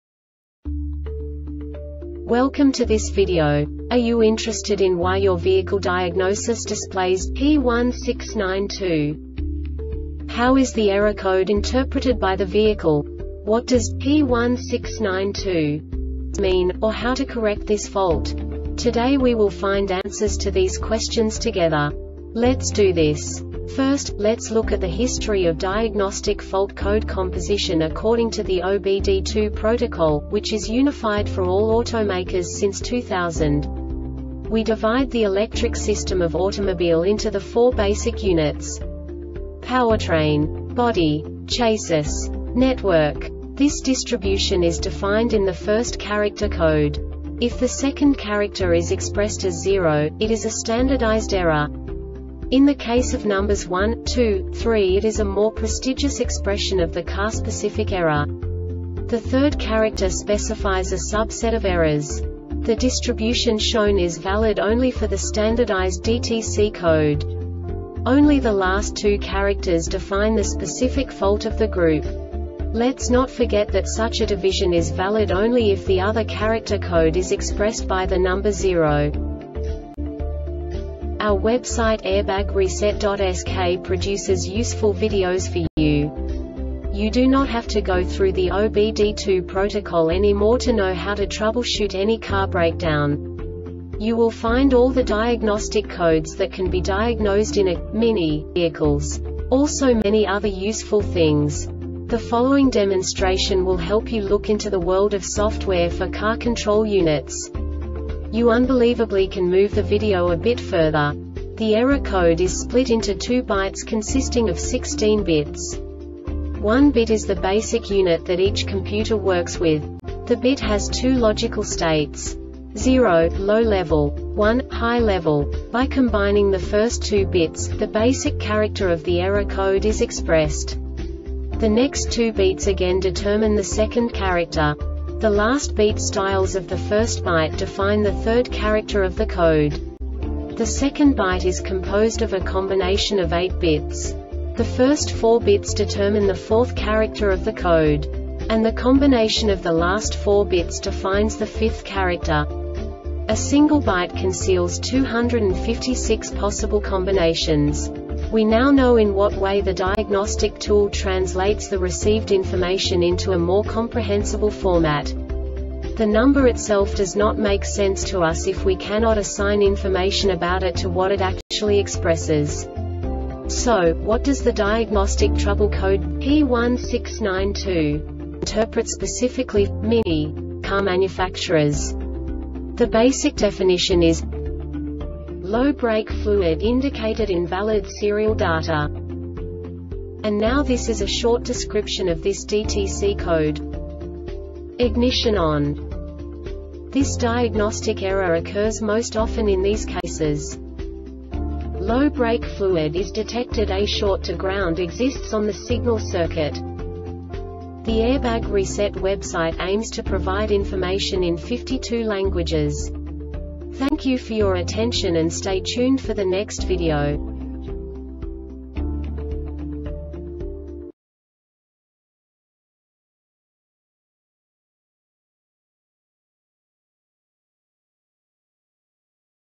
DTC Mini P1692 Short Explanation
Contents:
0:21 Basic DTC analysis according to OBD2 protocol standard.
1:48 Insight into programming
2:58 A diagnostically understandable description of the Diagnostic Trouble Code
3:05 Basic error definition

Make
MINI
Code
P1692
Definition
Low Brake Fluid Indicated Invalid Serial Data
Description
Ignition ON
Cause
Low brake fluid is detectedA short to ground exists on the signal circuit